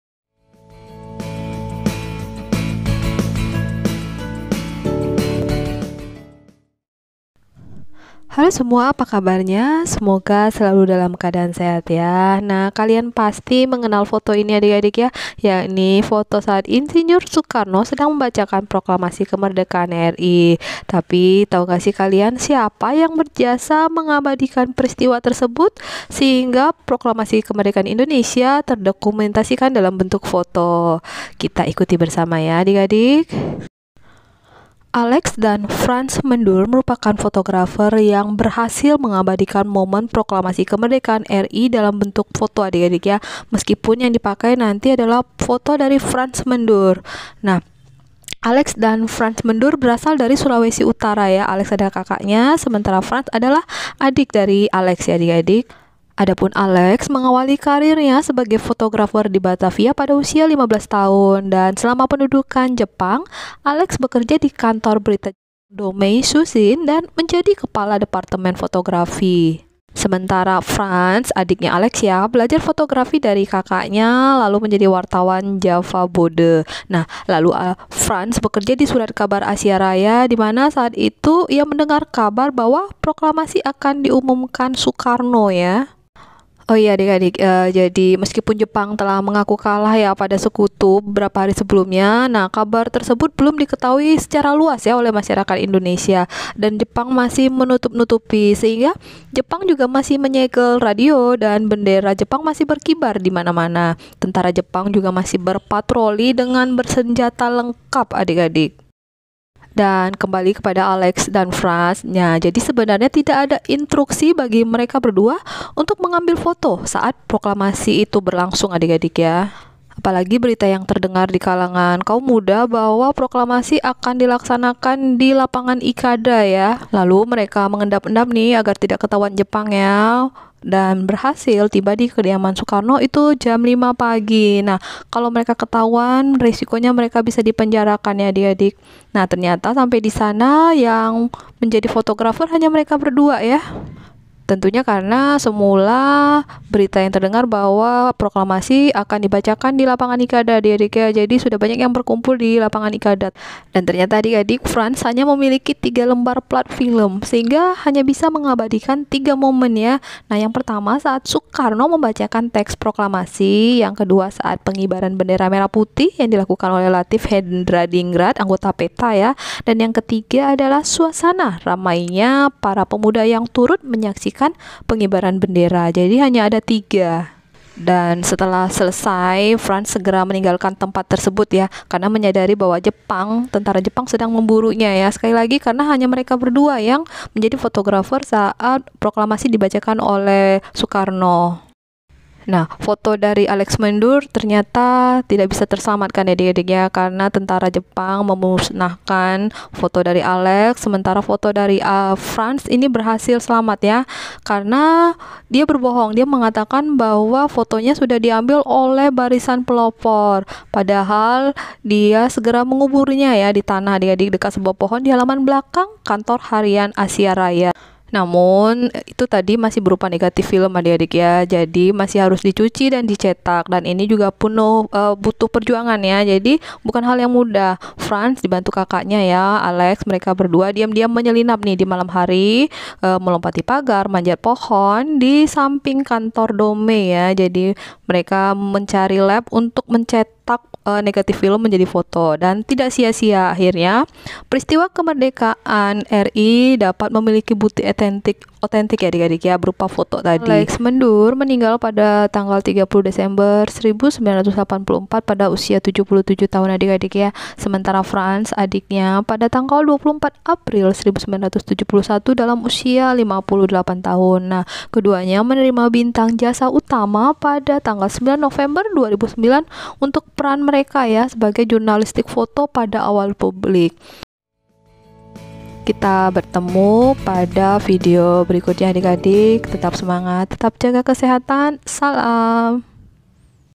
Alex Dan Frans Mendur - Fotografer Proklamasi Kemerdekaan RI
Alex Mendur dan Frans Mendur merupakan kakak adik yang berperan penting mengabadikan detik-detik Proklamasi 17 Agustus 1945.

Mereka berdua secara sembunyi-sembunyi mencetak foto tersebut dalam suasana ancaman tentara jepang

lengkapnya tonton di video ini ya